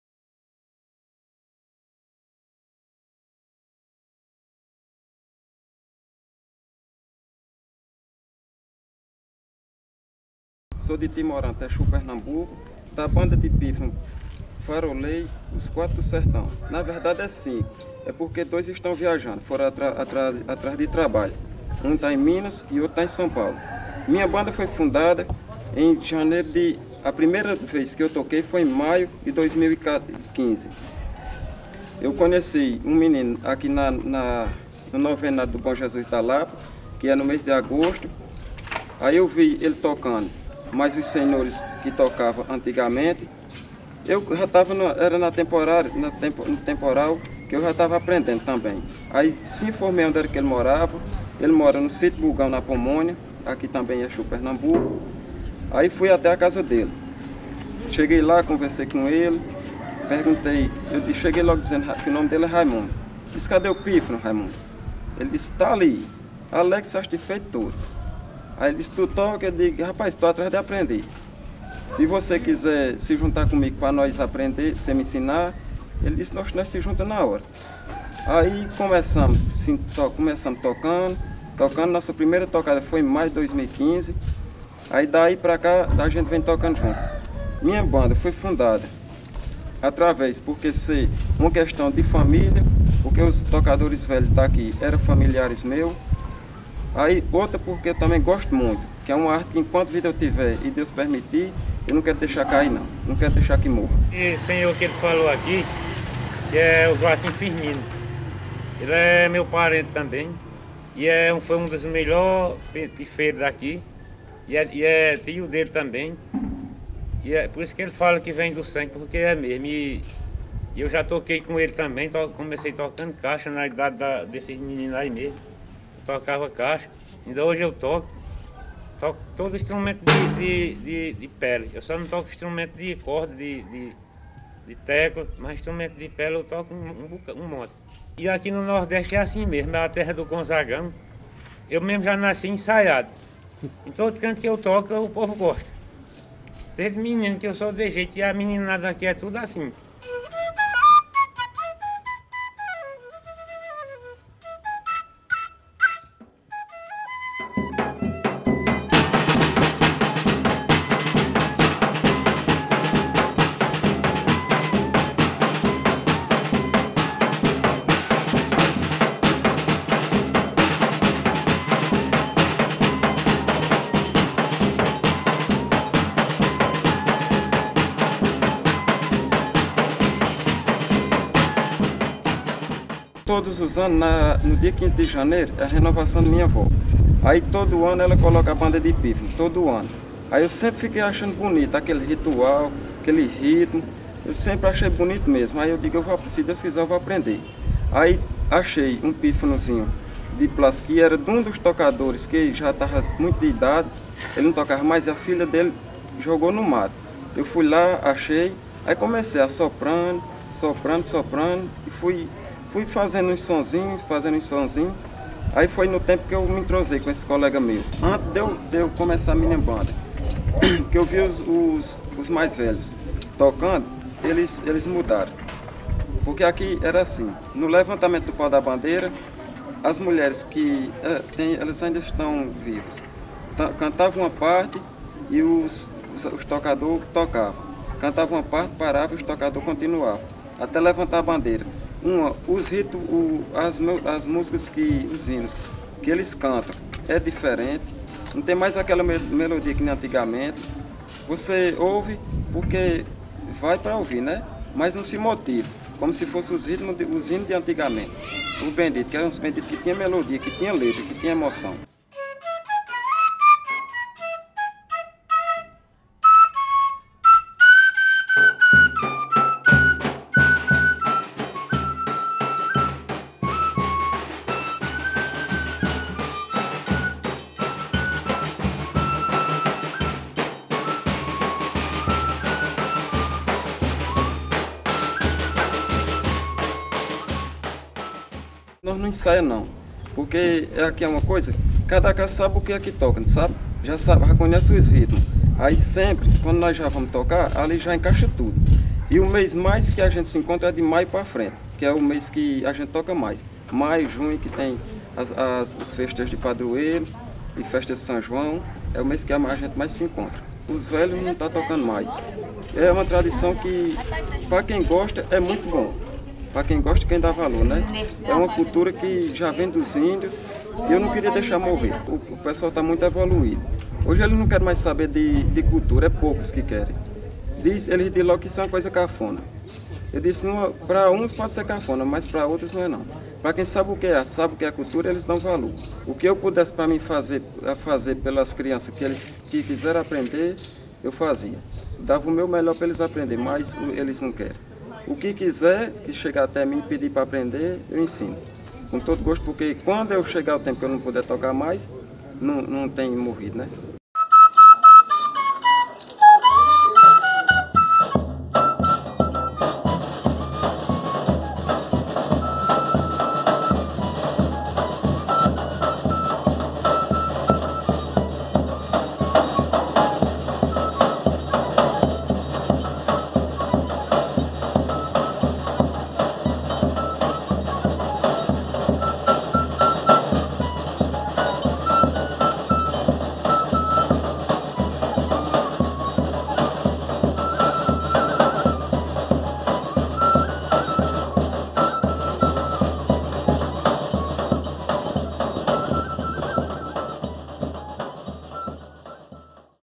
Banda de Pífanos Farolei Os Quatro do Sertão
Registro da Banda de Pífanos Farolei Os Quatro do Sertão, de Exu, em Pernambuco. Integra a o levantamento sobre bandas de pífanos pernambucanas, realizada pela Página 21.